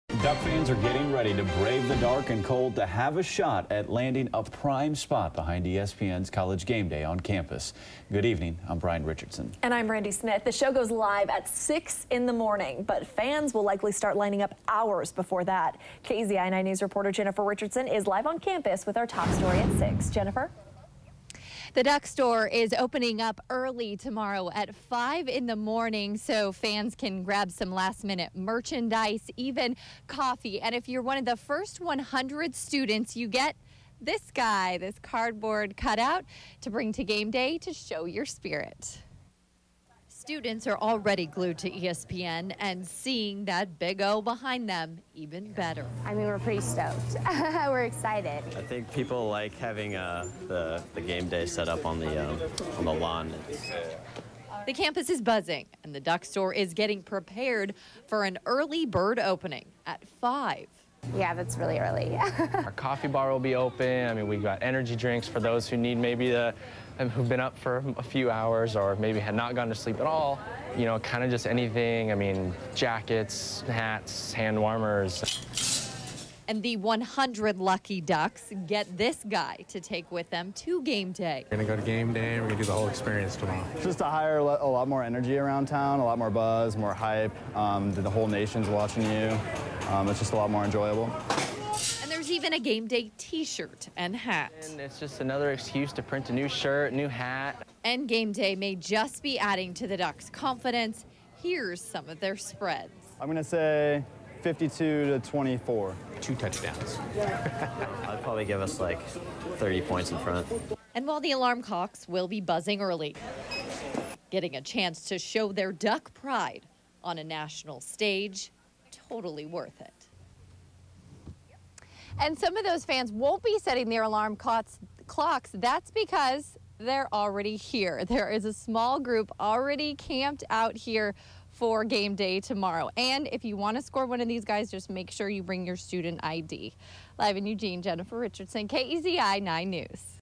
College GameDay Buzz on UO Campus
Duck fans are getting ready to brave the dark and cold to have a shot at landing a prime spot behind ESPN's College GameDay on campus. The show goes live at 6 a.m. on Saturday, but fans will likely start lining up hours before that.